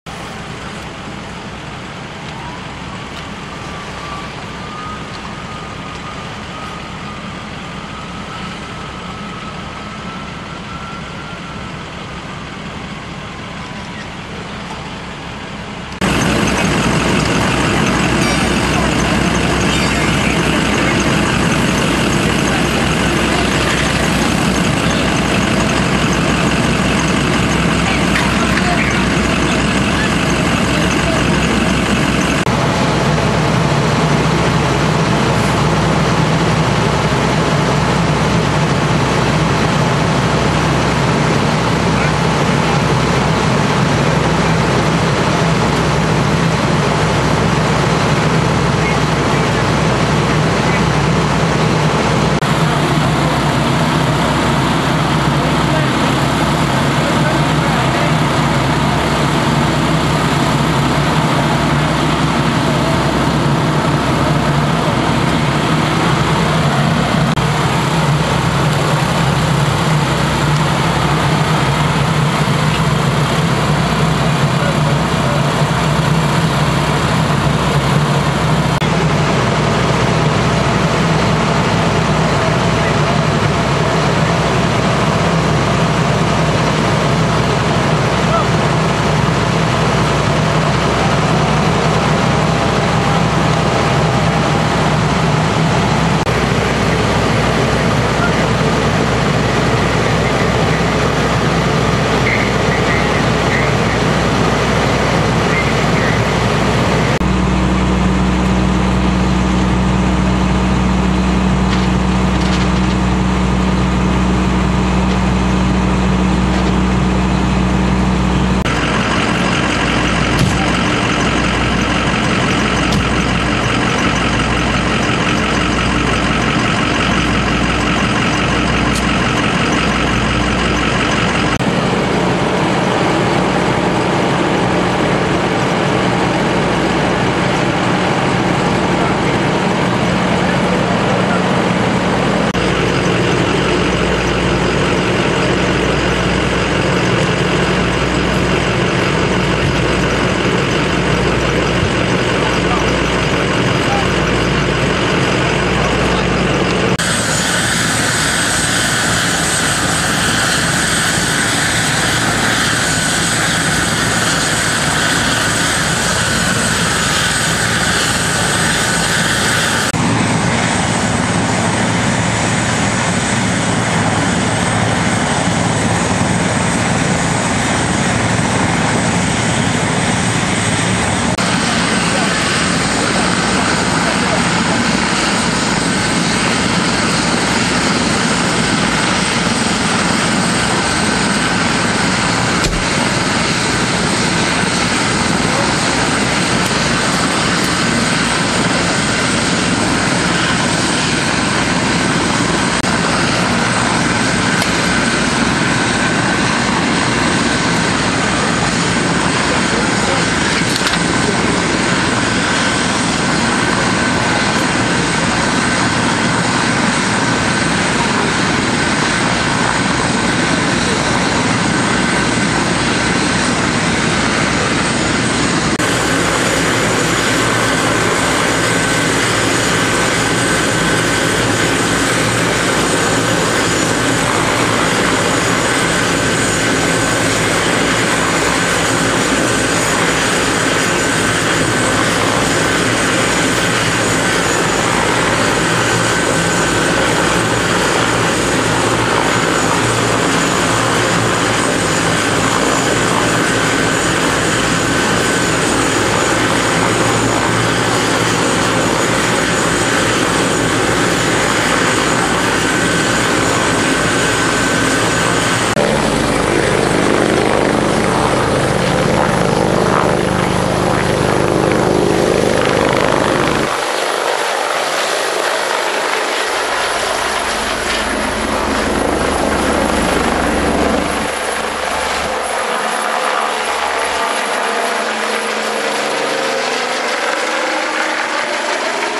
MVC Pin - Smithfield (2-23-2014)
MVC Pin - 500 blk NC 210, Smithfield (NC)
Smithfield Fire, Johnston County EMS, Wilson's Mills Fire, Smithfield PD
Duke Life Flight
3 patients (2 transported by ground to WakeMed and 1 transported by air).